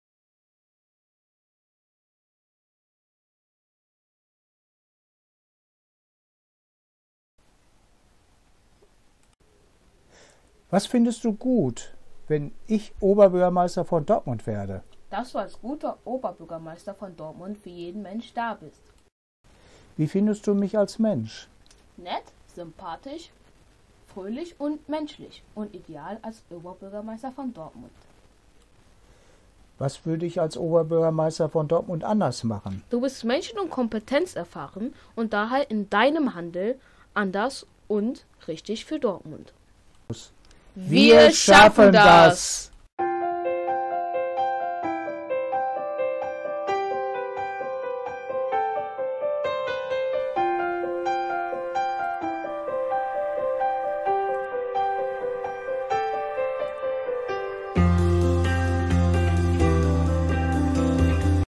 Wir schaffen das...
Kommunalwahl 2020!
Gutes tun, Mensch sein... und das anders... Ingolf Griese Oberbürgermeisterkandidat im Dialog mit Maurice